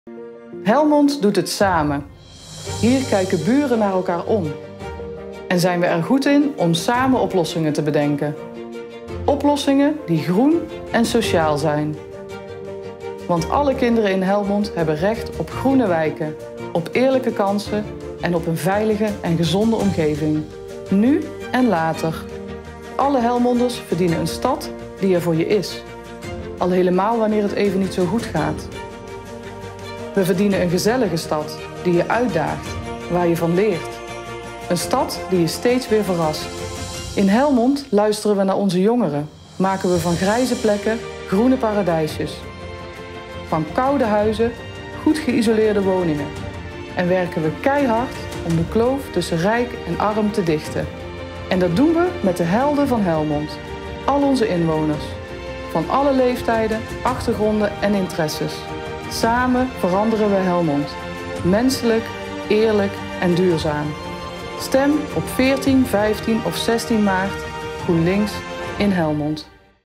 Laat je stem horen voor HELMOND | Gemeenteraadsverkiezingen 2022 | GroenLinks
Stem 14, 15 of 16 maart GroenLinks voor een menselijk, eerlijk en duurzaam Helmond. ❤️💚